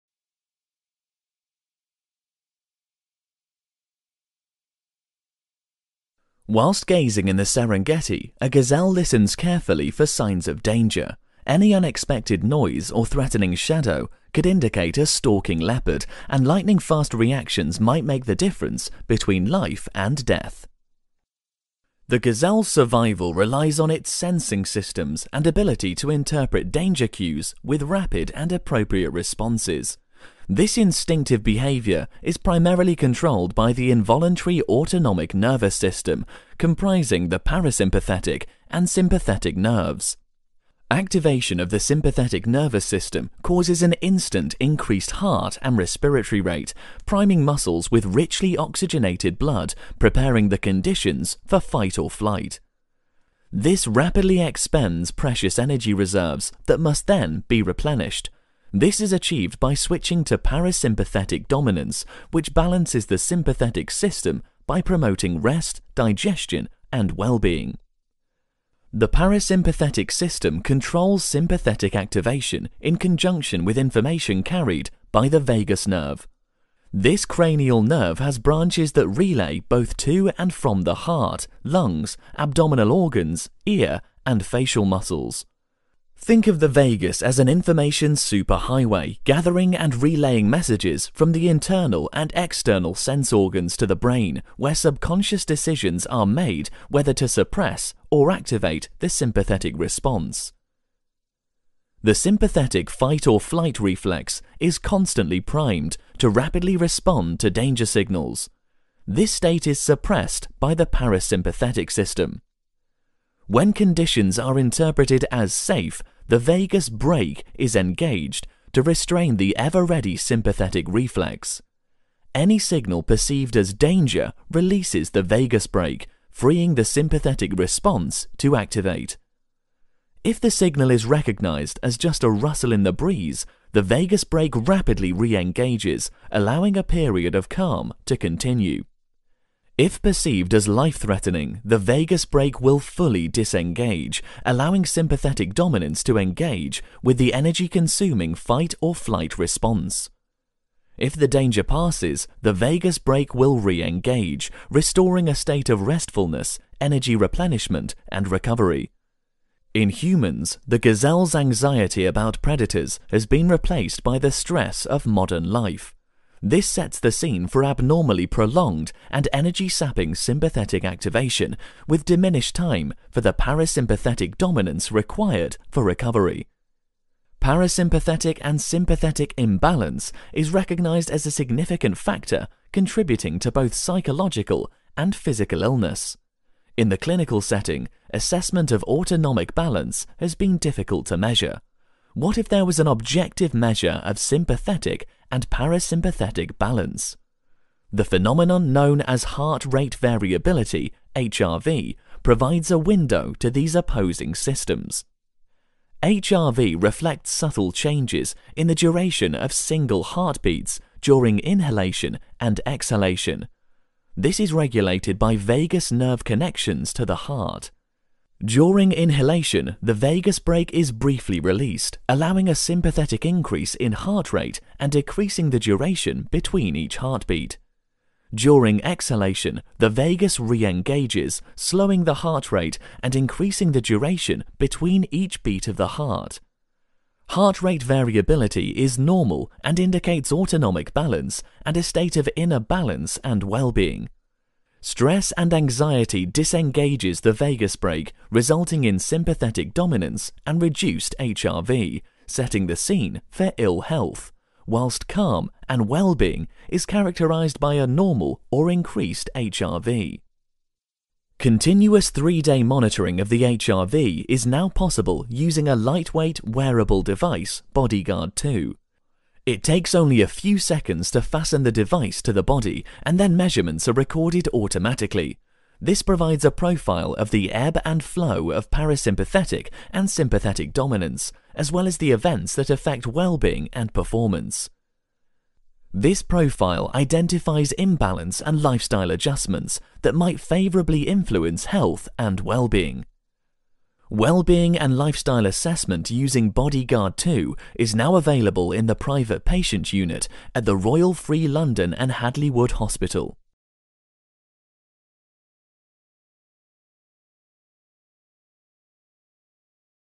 Heart rate variability
This videoscribe explains how the vagus nerve regulates the fight and flight response and how measurement of heart rate variability provides a  moment to moment window on the state of vagus influence on the "unconscious" autonomic nervous system. This, in turn, provides  measurable physiological insights to "inner balance" and "wellness". Loss of normal heart rate variability  is an early warning sign of both psychological and physical ill health. Continuous 72 hour heart rate variability can be measured using a lightweight wearable device (Bodyguard 2) providing individuals with physiological information on vagus tone, stress (good and bad) and, perhaps most important, recovery and restoration. The videoscribe  explains the importance of the vagus in modulating emotional and physical well being. This neurophysiology has only recently been described by Professor Stephen Porges in his work "The Polyvagal Theory" and although not yet taught to healthcare professionals, measuring and monitoring the vagus nerve is likely to become increasingly important to the understanding health, wellbeing and the factors contributing to ill health.